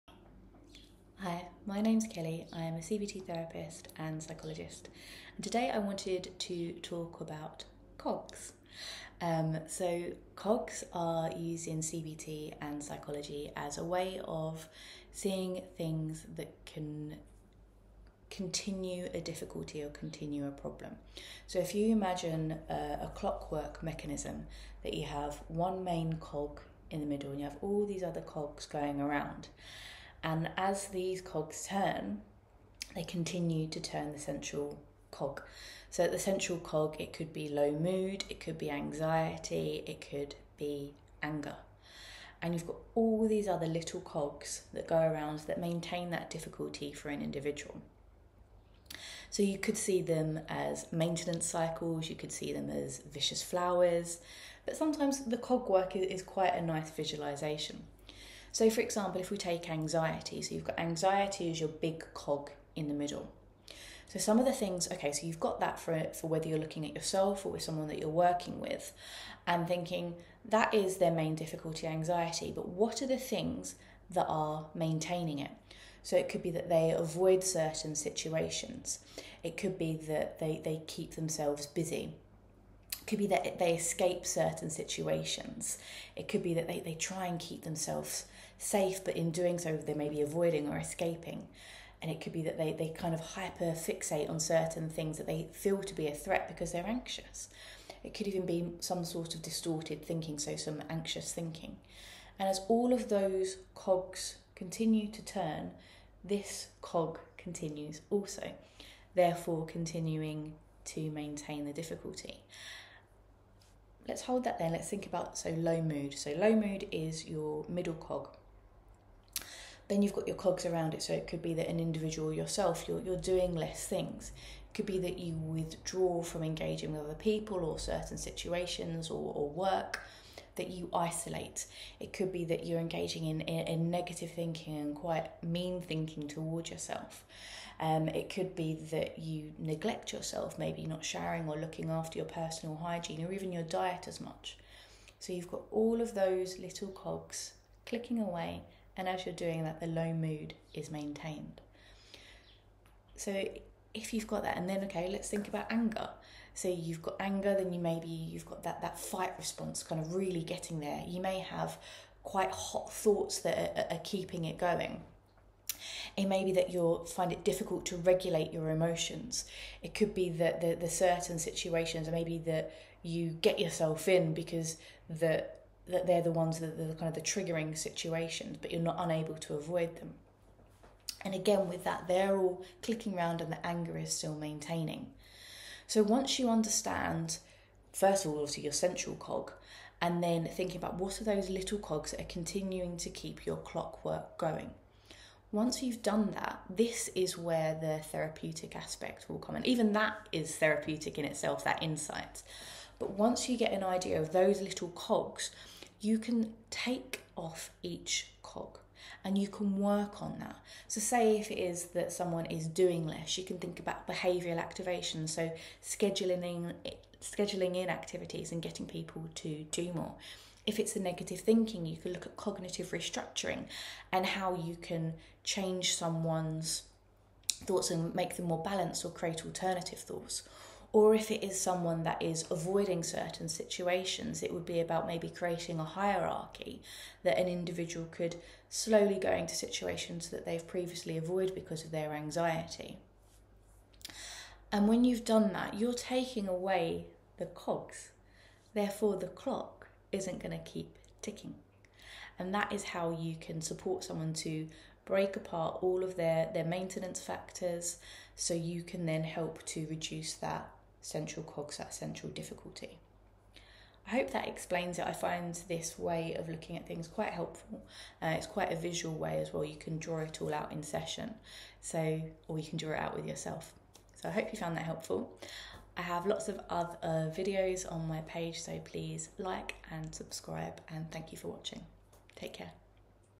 What keeps a problem going? Maintenance factors in formulation! Cogs, vicious flowers & cycles.
This way for formulating can be for you or someone else to see what are the things that keep your problem going.